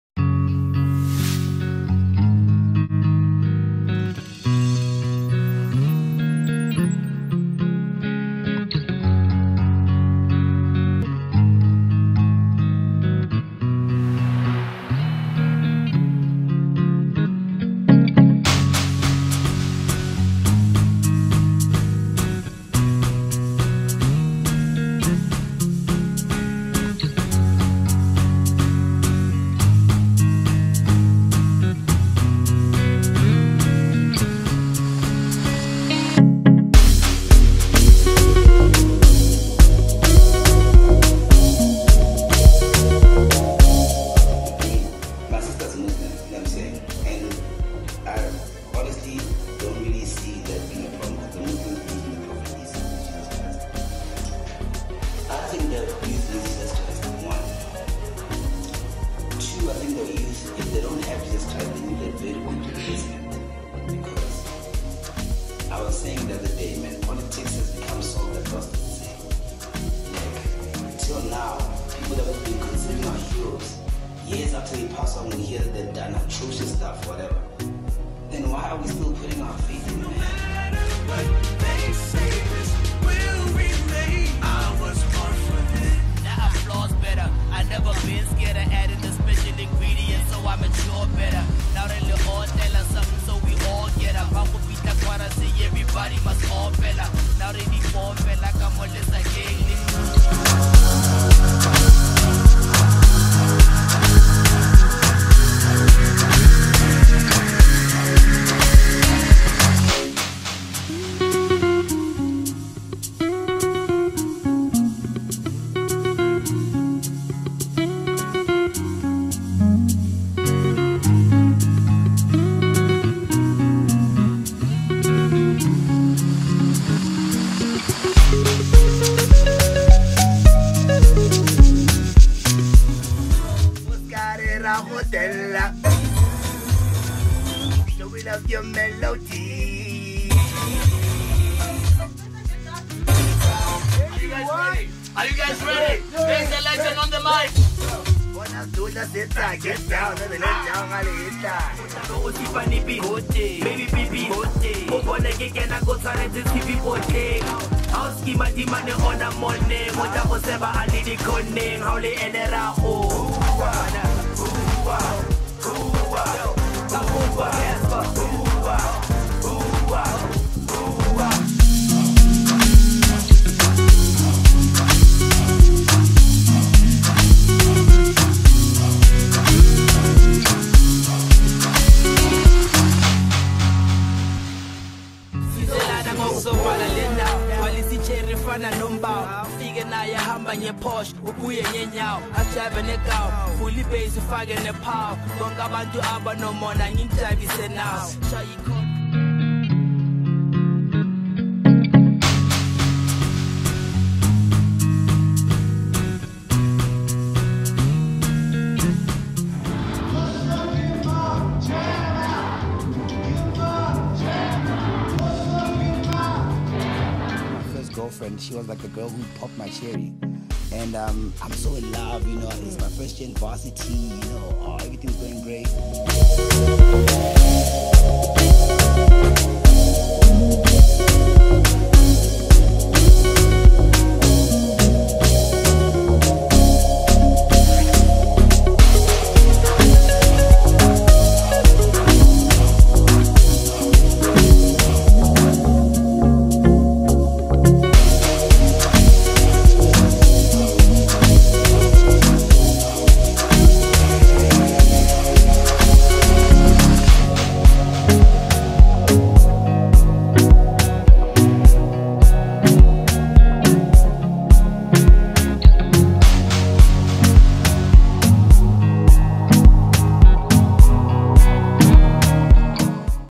HHP, JABBA (Official Tribute Video/With Prince Kaybee Tribute To HHP)
This is A Special Tribute Video, For The  Late Motswako Hip Hop Icon/Legend Jabulani Tsambo Popularly Known As HHP, JABBAMAN, Lenyora, Senganga & Le Pantsula Lao Shapa Hip Hop. In Loving Memory and Remembrance of HHP (14 September - 24 October 2018) we will always love you and remember you uncle Jabba❤️🇿🇦 your Legacy Lives on Forever.